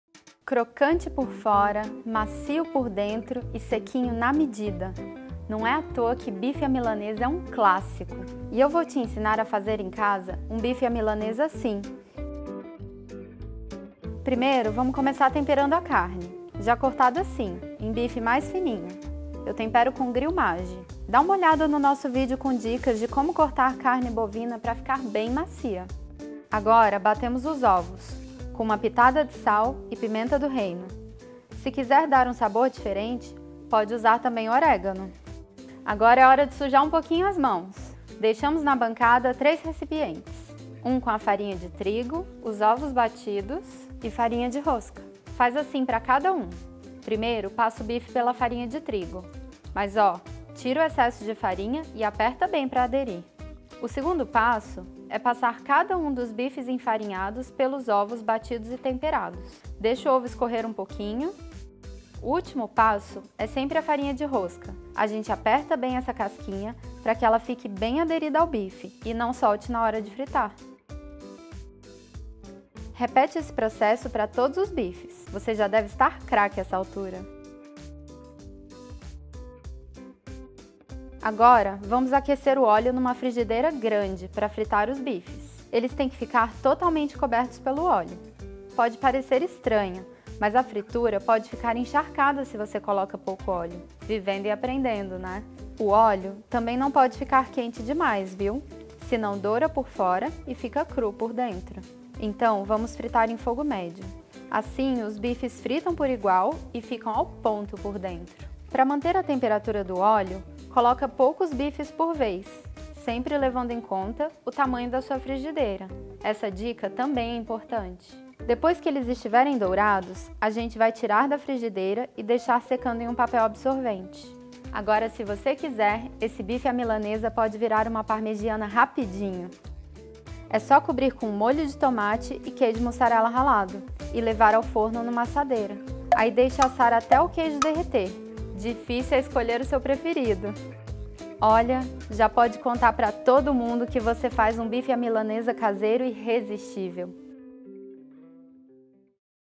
Bife à Milanesa - Receitas Nestlé
Bife temperado com Gril MAGGI®, envolvido em uma camada de ovos e farinha rosca e fritos em imersão. Acesse essa receita no nosso portal